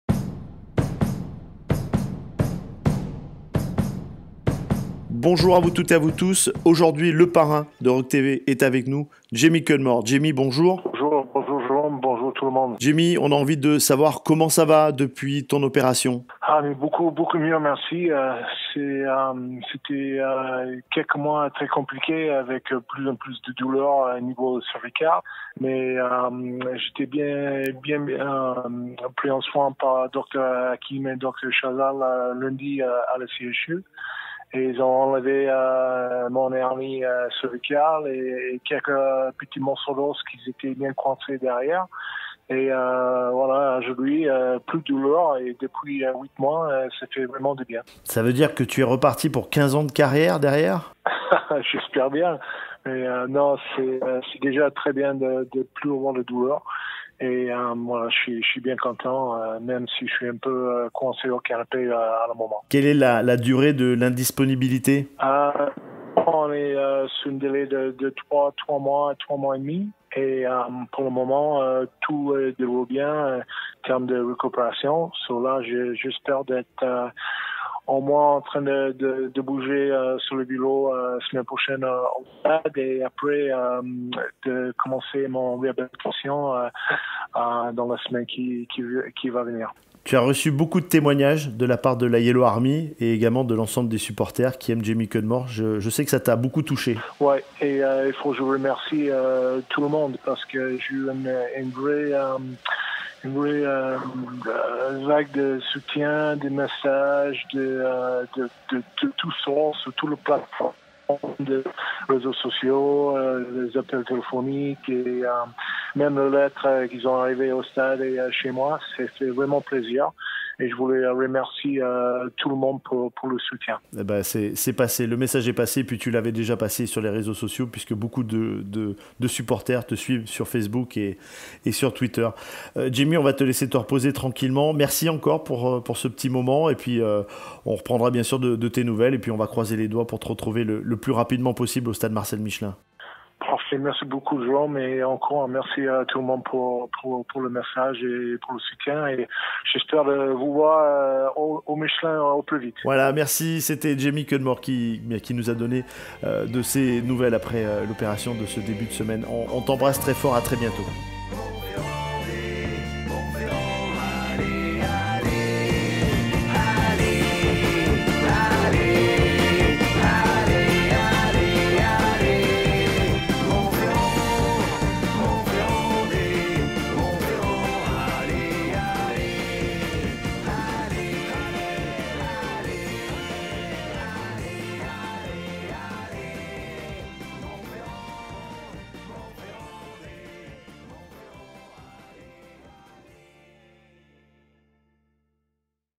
Jamie Cudmore nous donne de ses nouvelles après son opération
Jamie Cudmore nous donne de ses nouvelles après son opération, et a un petit mot pour la YellowArmy pour tous les messages de soutiens qu'il à reçu.